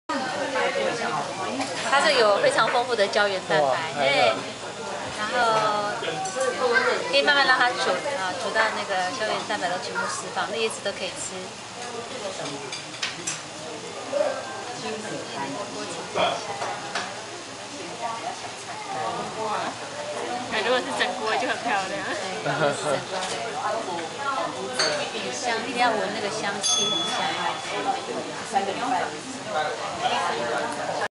櫻之田野養生野菜鍋～蓮花在鍋中盛開
在櫻之田野養生野菜鍋，
體驗蓮花在鍋中盛開。